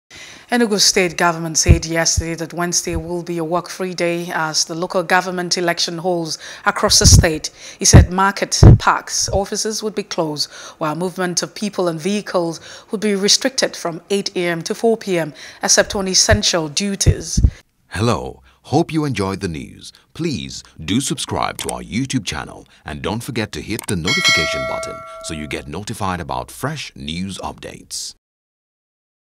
Enugu Local Poll: State Declares Feb.23 Work Free, To Shut Down Businesses | NEWS
Enugu State Government said yesterday that Wednesday will be a work-free day, as the local government election holds across the state.